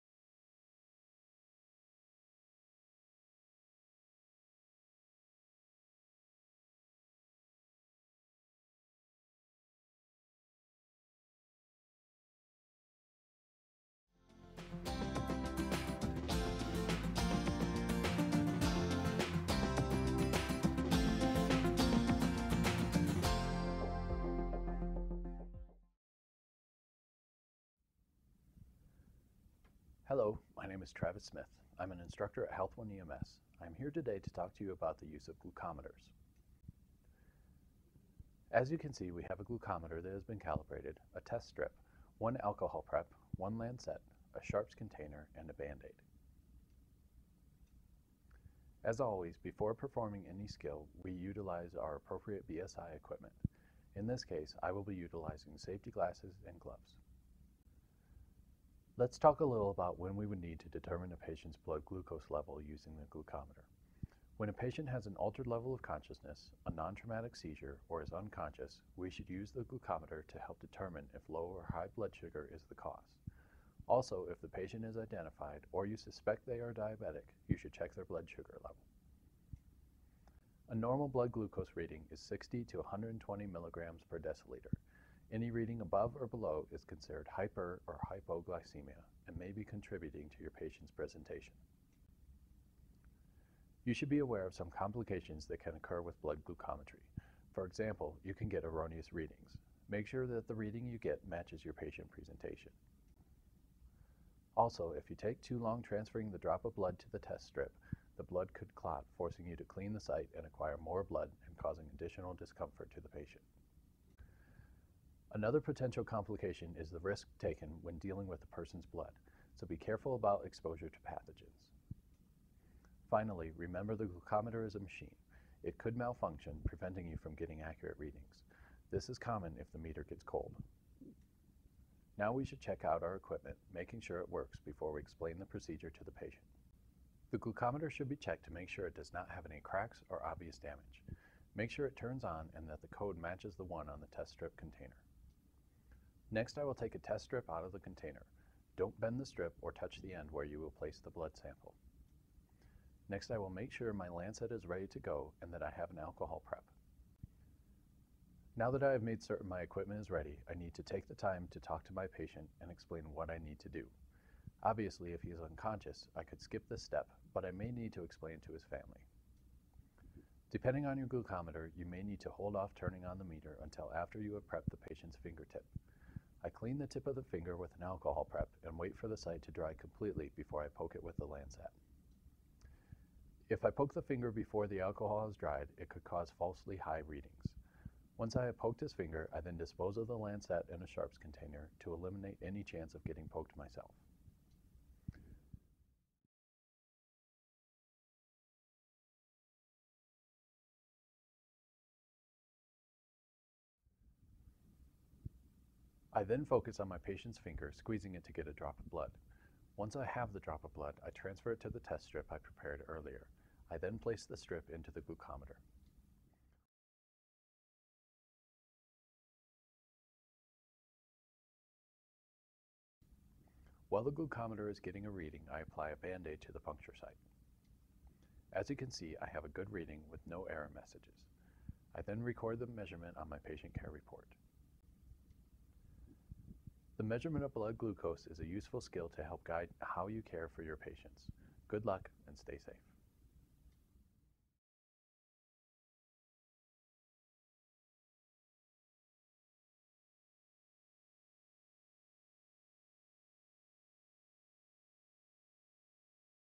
Glucometry
HealthONE EMS EMT Skills Series.  This video demonstrates the proper use of glucometry in patient care